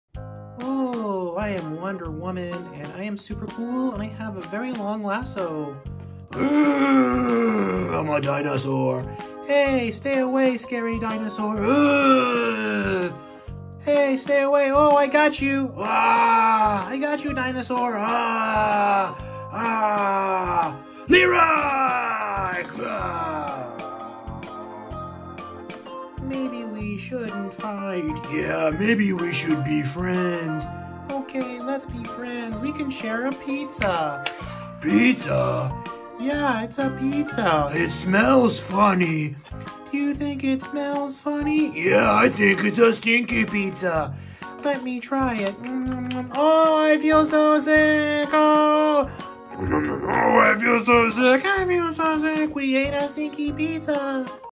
Wonder Woman Meets a Dinosaur and They Share a Stinky Pizza
Wonder Woman Meets a Dinosaur and They Share a Stinky Pizza. Super cool:)